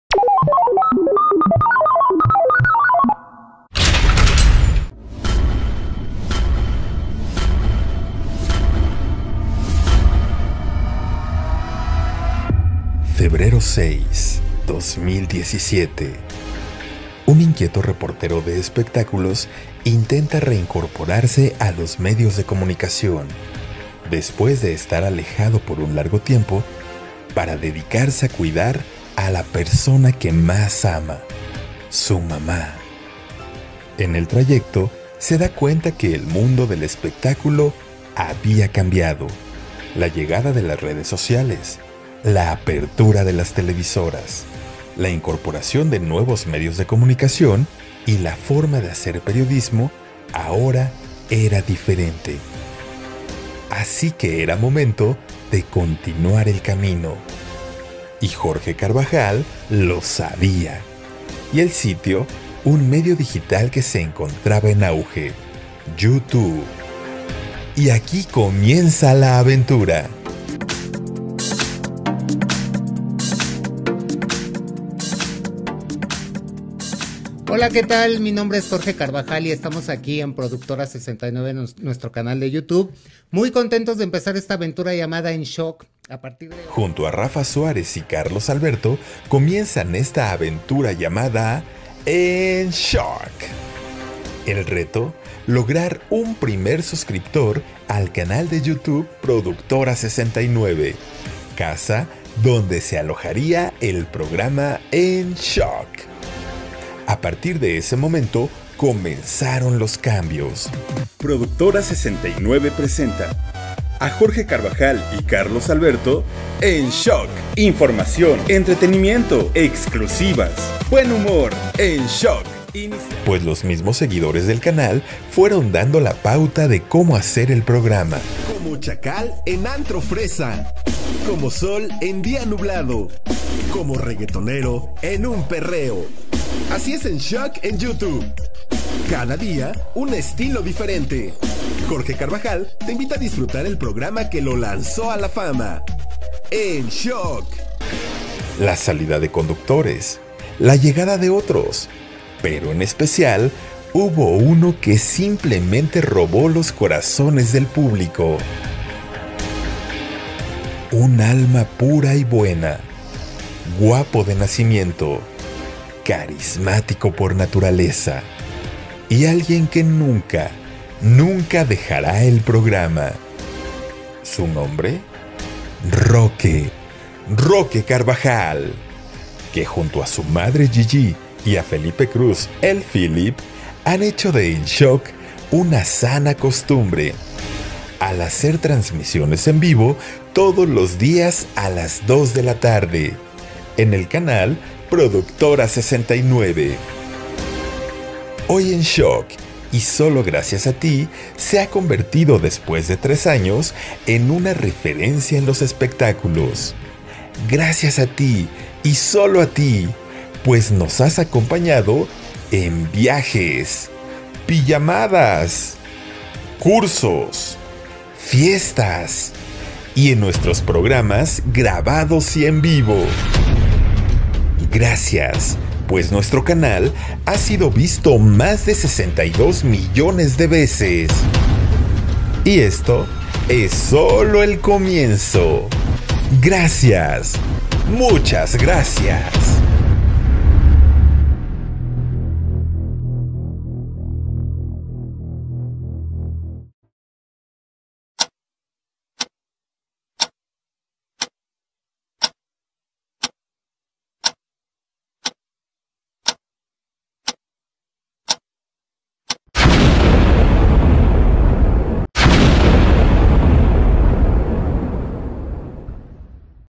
¡TRES AÑOS DE ... EN SHOCK!
Tres años compartiendo con ustedes toda la información del mundo del espectáculo, anécdotas, viajes y y habladas de todo y contra todos, gracias por el apoyo y el cariño de siempre!.